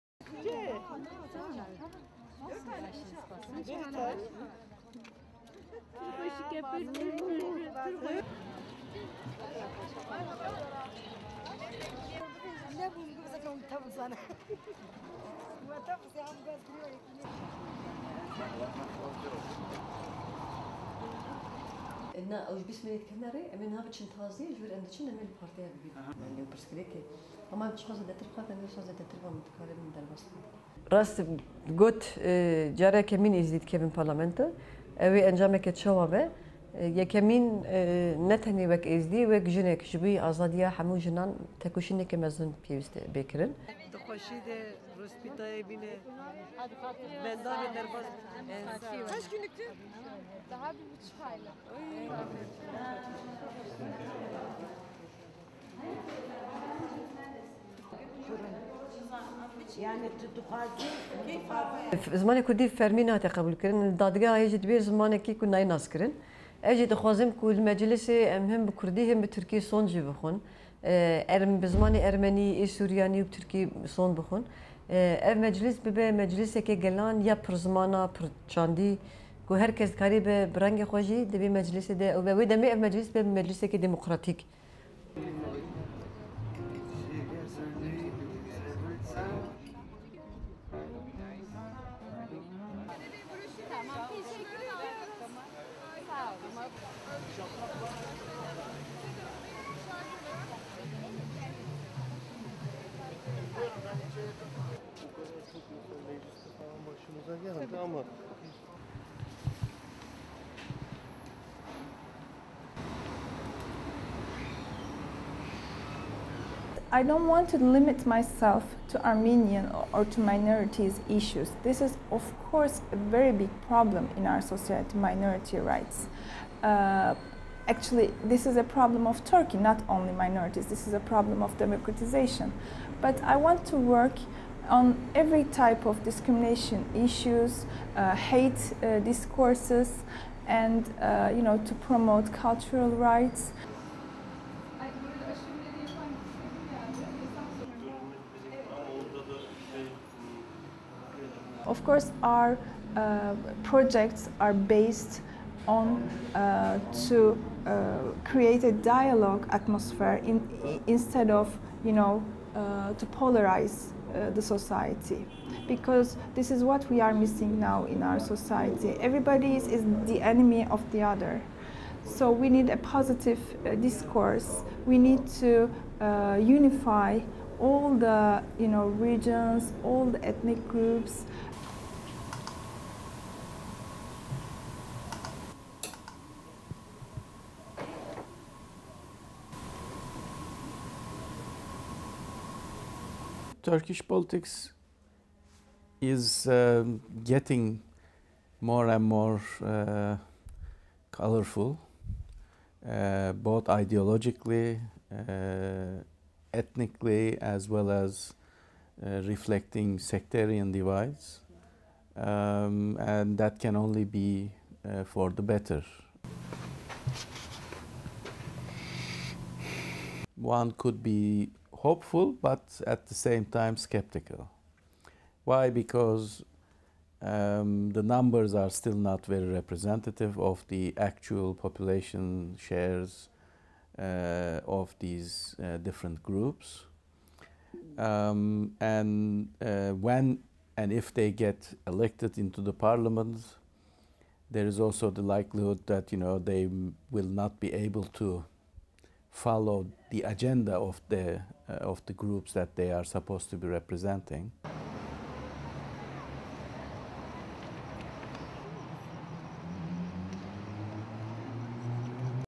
Minority candidates stand in general election
(25 May 2015) LEAD-IN:
It's less than two weeks to Turkey's general election and minority candidates are campaigning hard for a seat in parliament.
The candidates include the first Yazidi and the first female Armenian ever to stand for election in Turkey.

STORY-LINE:
Parliamentary candidate Feleknas Uca cradles a baby on the campaign trail.
It's typical behaviour for a prospective member of parliament trying to garner votes. But Uca is not a typical candidate.
She's the first Yazidi person to stand for election in Turkey and is running in Diyarbakir for the pro-Kurdish People's Democratic Party, or HDP.
The close-knit Yazidi community are one of Turkey's smallest minorities.
But the numbers increased last year when thousands of Yazidis sought refuge in Turkey from persecution by Islamic State group militants in Iraq.
Uca says she will not be limited to working for just her own ethnic community if she is elected.
"I have been asked what it means for Yazidis to be represented in Parliament. For me, more than that, our priority is going to Parliament and representing women in general," she says.
Today Uca is meeting local women to talk with them about their rights.
She's a seasoned politician and was the first Yazidi to be elected to the European Parliament in 1999.
Uca is not a fluent Turkish speaker - she was born in Germany and her family only taught her Kurdish.
But she thinks there is room for many languages in the country's parliament.
"We would like to make our oath in the Kurdish language, in the Turkish language, as well as in Armenian, Syrian and other languages in the Parliament," she says.
"This way we will practise the representation of all these languages in the parliament."
Other political parties are also embracing minority candidates.
Campaigning in Istanbul, Selina Ozuzun Dugan is the first female Armenian to run for parliament.
She's the top candidate for the main opposition party, the secularist Republican's People Party, or CHP, in Istanbul's second electoral district.
Armenians are also running for other parties: the HDP is fielding three Armenian candidates and the ruling Justice and Development Party (AKP) party has one.
Armenians have long campaigned for recognition that the World War I killings of their ancestors in Turkey constituted genocide - something the country's government fiercely lobbies against.
But Dugan plans to work on a wide variety of issues.
"I don't want to limit myself to Armenian or to minority issues. This is of course a very big problem in our society, minority rights. Actually this is a problem of Turkey, not only minorities, this is a problem of democratisation," she says.
"But I want to work on every type of discrimination issues, hate discourses, and you know, to promote cultural rights," she continues.
Dugan is aware that bridges need to be built between communities.
"Our project is based on to create a dialogue atmosphere instead of to polarise the society, because this is what we are missing now in our society. Everybody is the enemy of the other," she says.
"So we need a positive discourse. We need to unify all the regions, all ethnic groups."
Political science professor Ali Carkoglu believes the minority candidates will have a positive impact.
"Turkish politics is getting more and more colourful, both ideologically, ethnically, as well as reflecting sectarian divides and that can only be for the better," he says.
But he warns their influence is likely to be limited.
A Roma candidate is also running for the first time, while Greek candidates are standing for the first time since the 1940s.